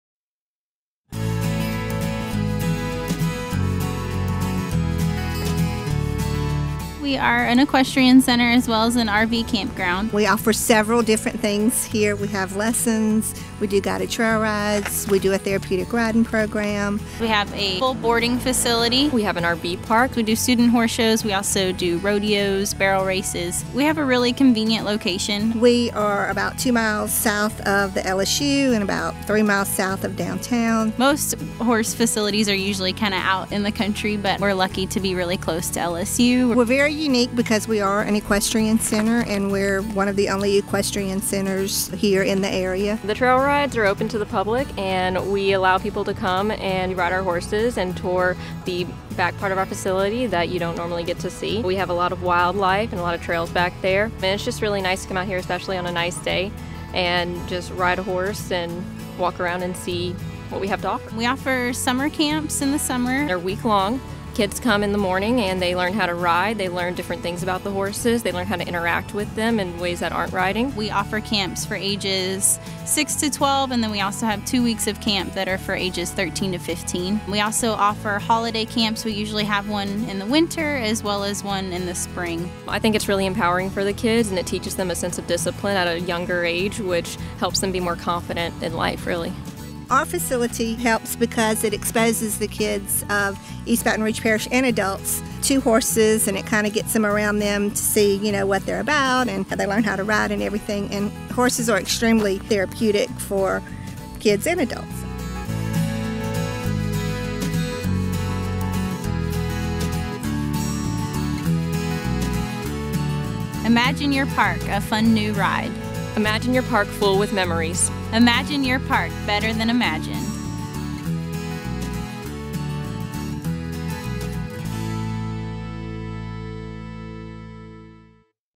BREC's Farr Park Equestrian Center and RV Campground
BREC's Farr Park Equestrian Center and RV Campground is one of the areas largest and most versatile centers for horse enthusiasts. It features a newly renovated indoor arena and an outdoor arena (both lighted), 256 stalls, a cross-country event course, horse trails and RV campground.
The facility is located only two miles from the LSU campus and six miles from downtown Baton Rouge attractions. The campground is adjacent to the Mississippi River, located on historic River Road. It is easily accessible from Interstate 10. The campground has 108 sites (many pull-throughs) and is equipped with water and power hookups and restroom facilities.